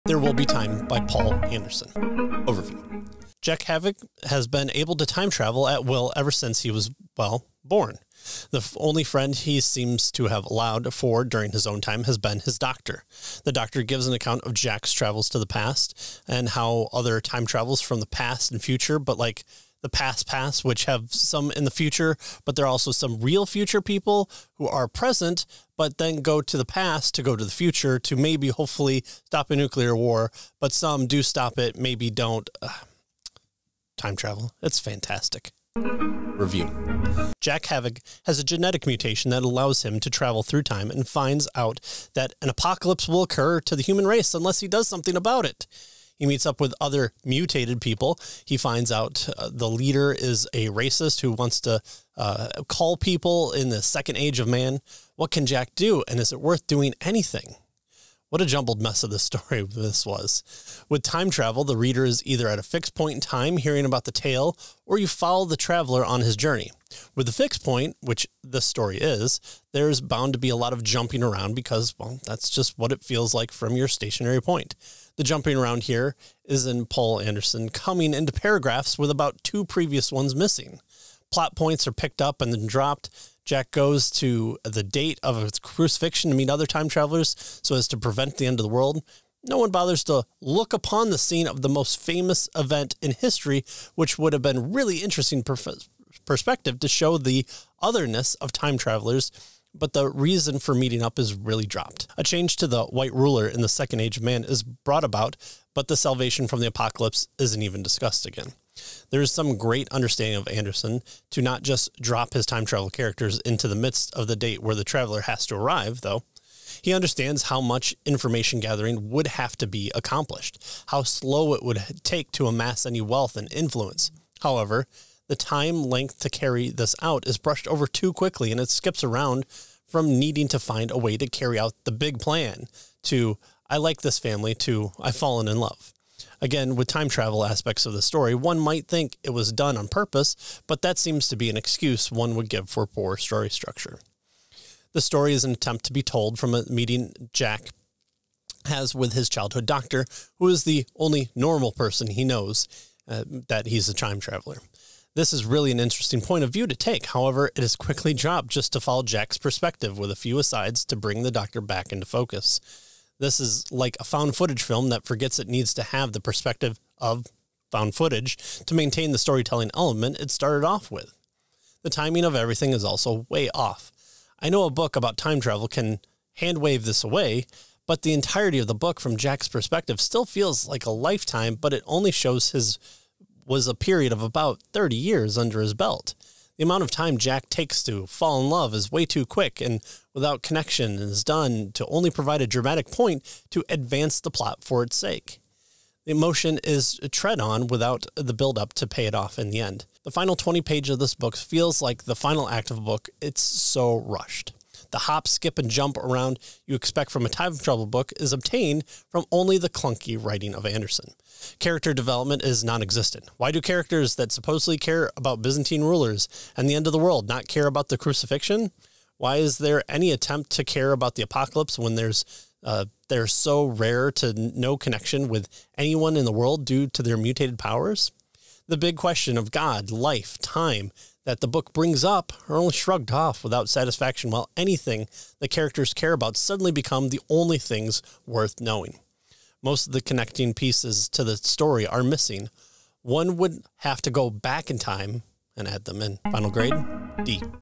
Book Review - There Will Be Time by Poul Anderson
Jack Havig has been able to time travel at will ever since he was well, born. The only friend he seems to have allowed for during his own time has been his doctor. The doctor gives an account of Jack’s travels in the past and how other time travels from the past and future but like, the past’s past which have some in the future but there are some real future people who are present but then go to the past to go to the future to maybe, hopefully, stop a nuclear war but some do stop it maybe don’t. Ahh time travel - it’s fantastic.